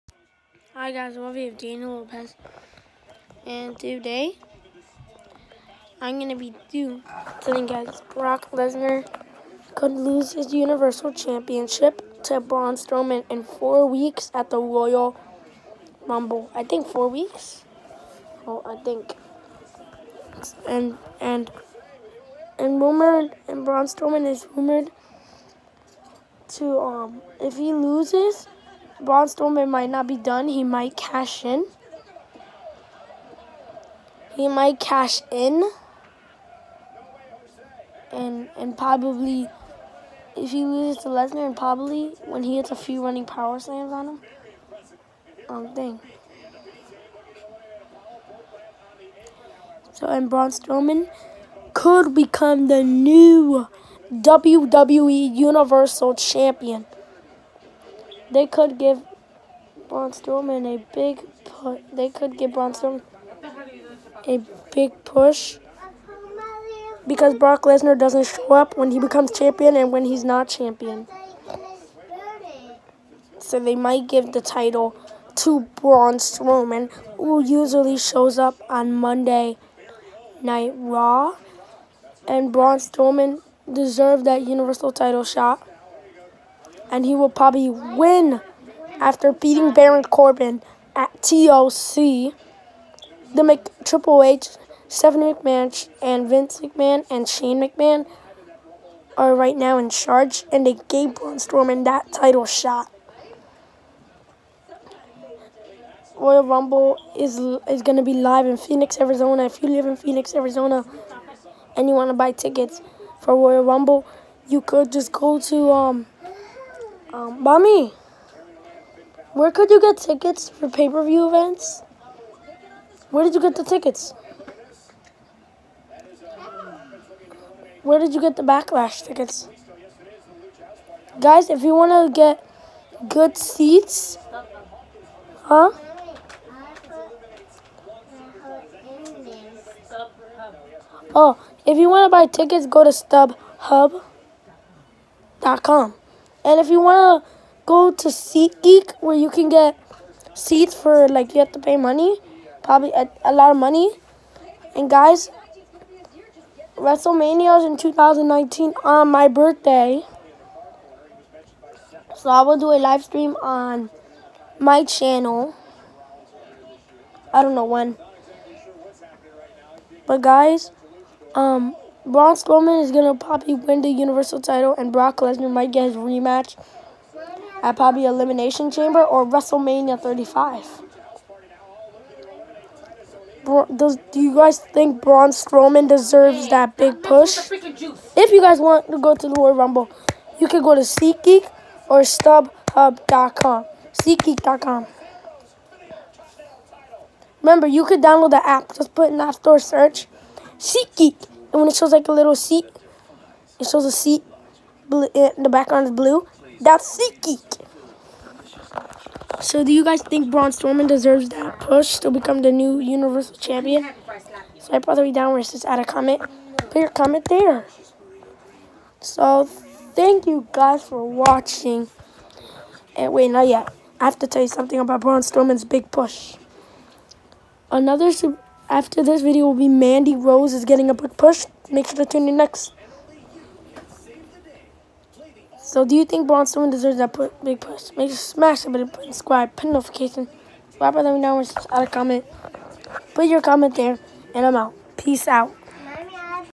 Do you think Bruan Strowman Deserves a big push to become the new universal champion
Bruan Strowman What does universal title shot at Royal rumble 2019 what do you guys think about that? Will Bruan Strowman win the universal title?